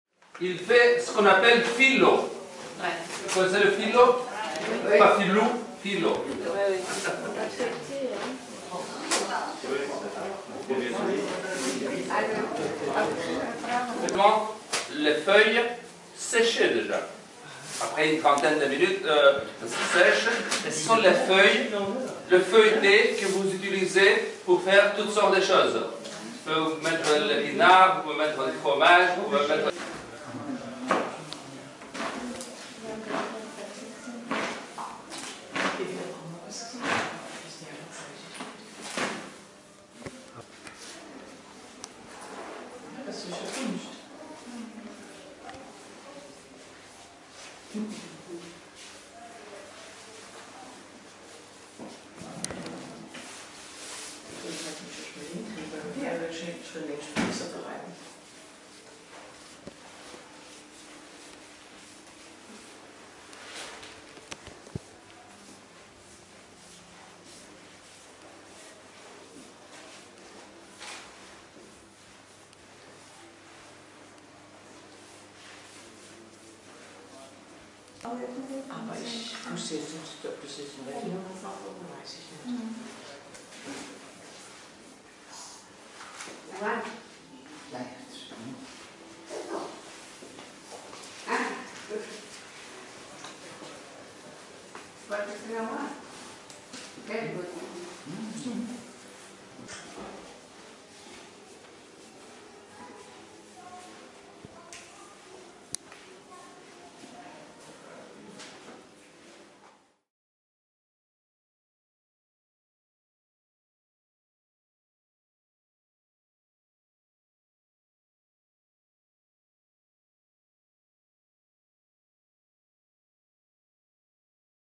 Giorgos Hatziparaskos - The Crete Icon Phyllo Dough Maker. A Legend!
We went back to see Giorgos Hatziparaskos , we visited him in 2016 in Rethymno on the Greek island of Crete and filmed him making his super thin, handmade phyllo do. Our video generated as for today over 120k views:

This video was actually shot in 2018 and we were wondering if he's still at it! And we're more than happy to report he is! Business Insider just went to visit and reported about him. He's still making his phyllo dough.

Enjoy our video and of course we captured him making his famous phyllo dough bubble.